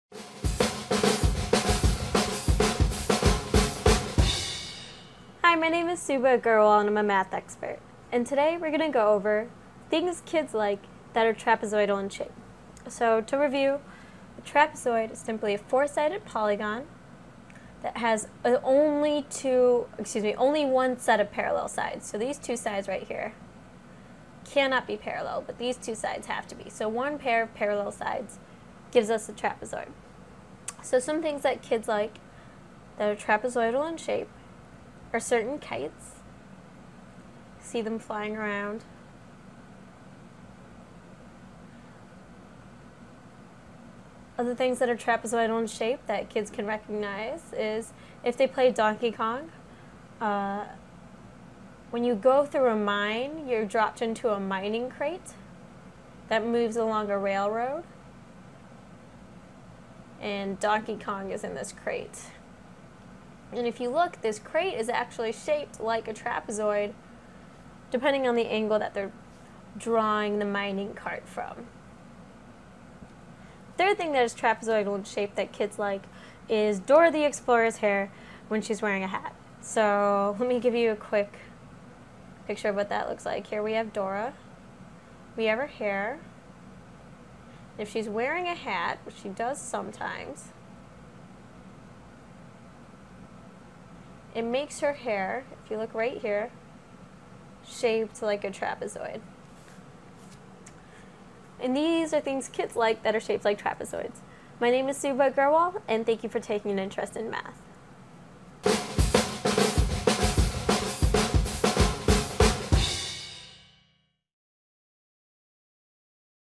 Things Kids Like That Are Trapezoidal in Shape
Things Kids Like That Are Trapezoidal in Shape. Part of the series: Fractions & Other Math Tips. If you want to start teaching kids about shapes, you should do so using objects that they are already familiar with. Find out about things kids like that are trapezoidal in shape with help from a mathematics educator in this free video clip.